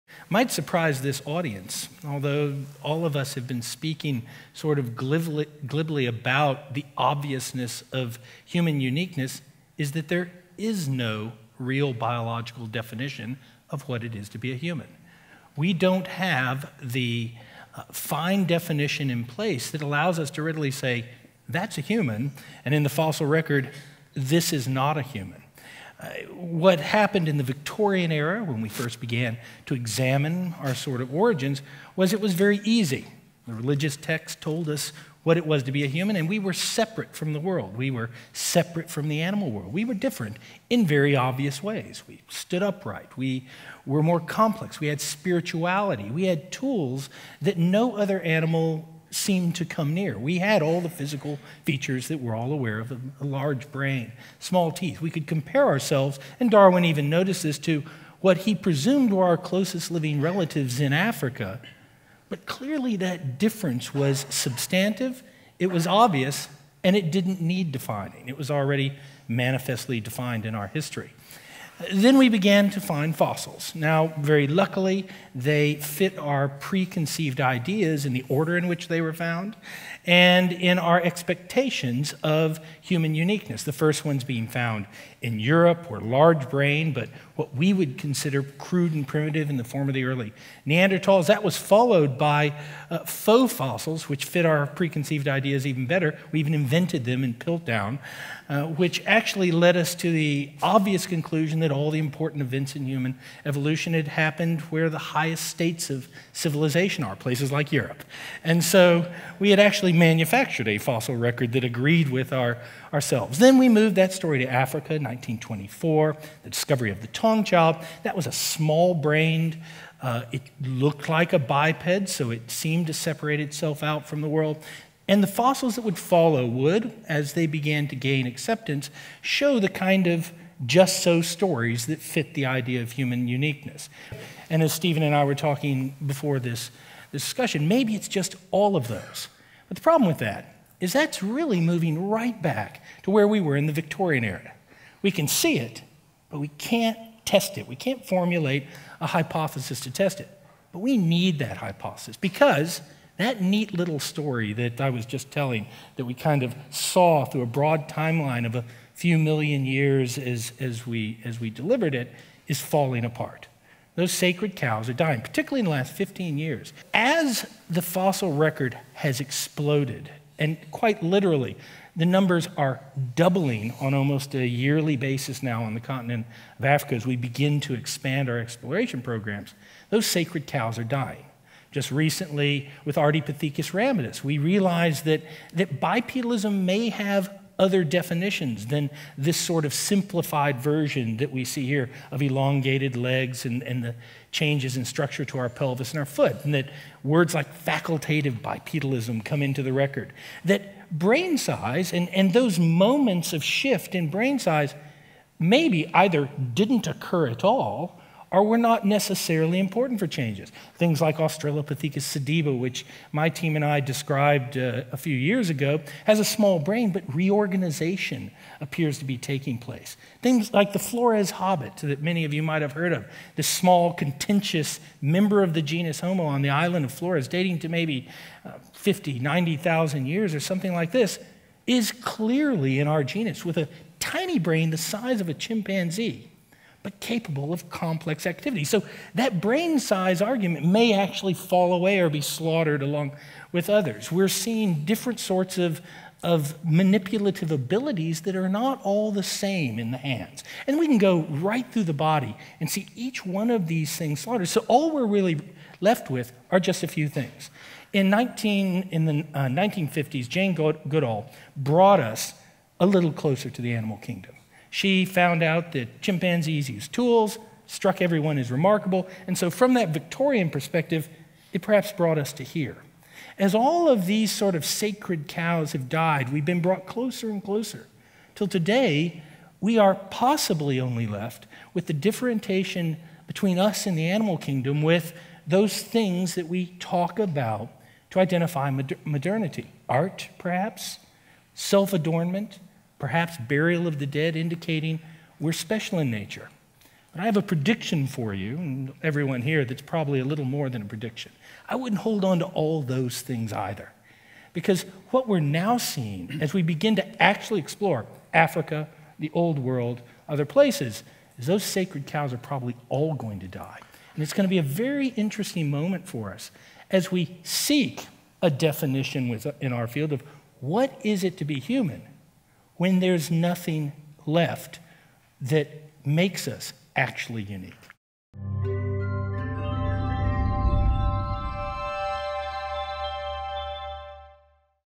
Who Says Humans Are Unique, Anyway?
While humans sure seem unique, there is no biological definition of human uniqueness, explains explorer and paleoanthropologist Lee Berger in this clip from the 2015 World Science Festival program “The Planet of the Humans: The Leap to the Top.” And as fossils continue to reveal more and more of the historical and evolutionary record, Berger argues, bit by bit the sacred cows of so-called human uniqueness (such as large brain size and bipedalism) are starting to die.